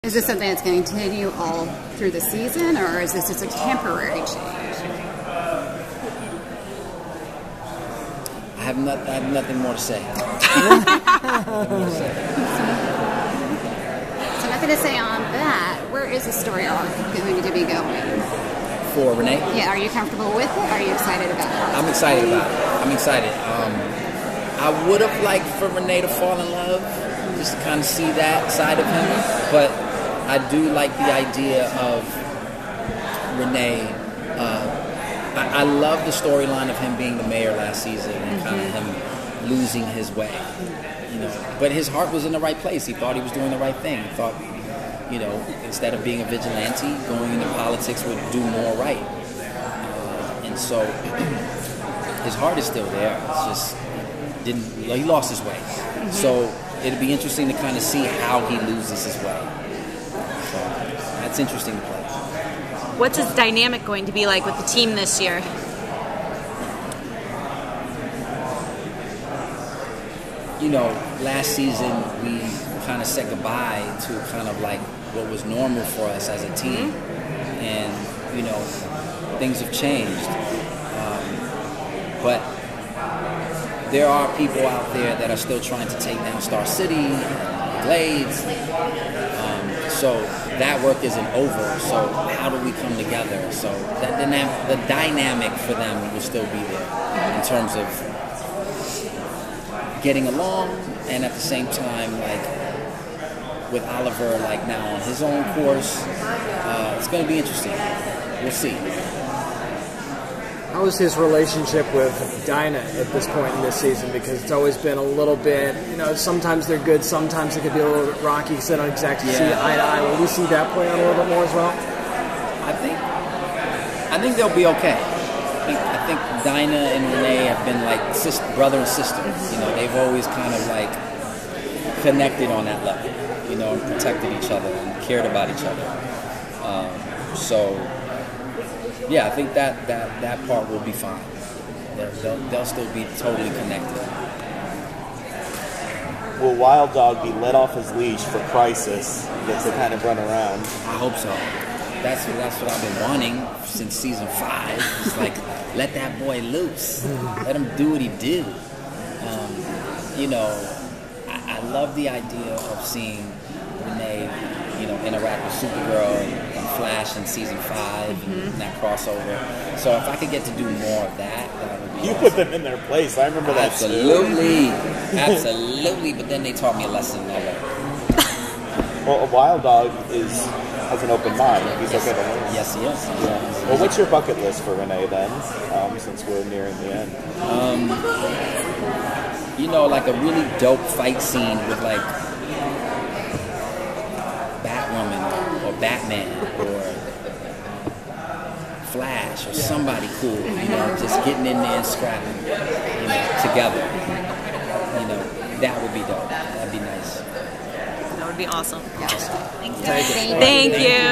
BB Exclusive: Rick Gonzalez Talks the Final Season of Arrow at SDCC
Rick Gonzalez talks what's to come for Wild Dog on the final season of Arrow at SDCC.